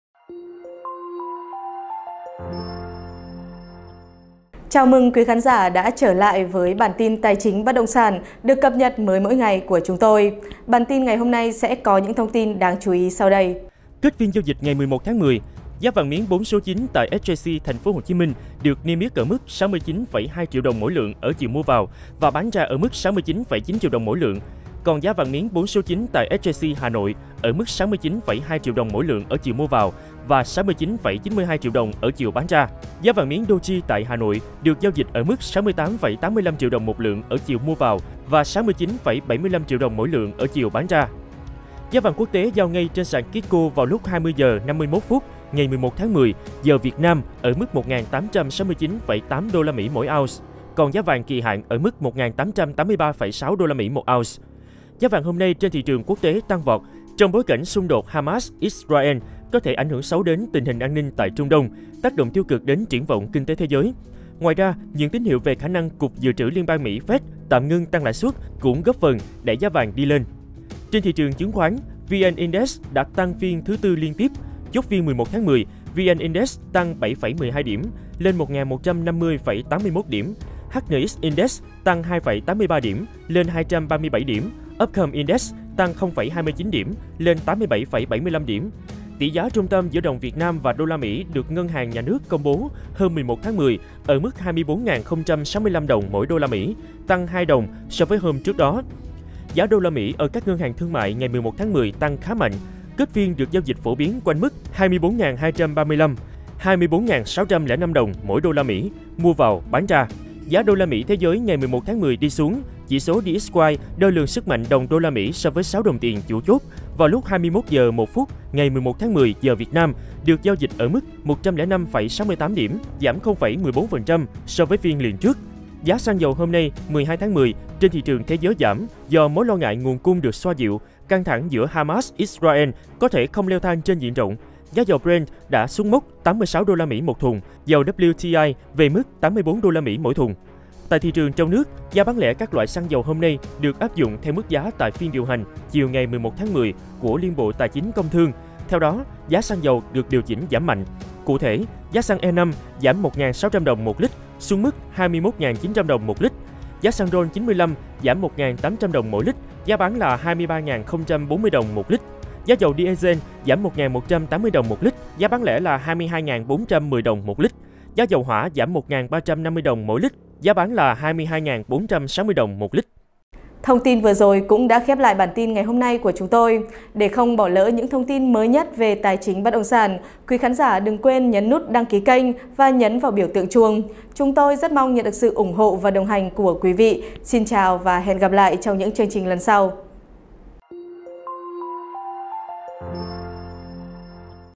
Giá vàng 12/10: Giá vàng tăng dữ dội, dầu và USD đồng loạt đi xuống | CafeLand
- Giá vàng trên thị trường thế giới tăng vọt
- Giá xăng dầu quốc tế có xu hướng tiếp đà giảm từ 2 phiên trước.
- Giá USD thế giới cũng đi xuống.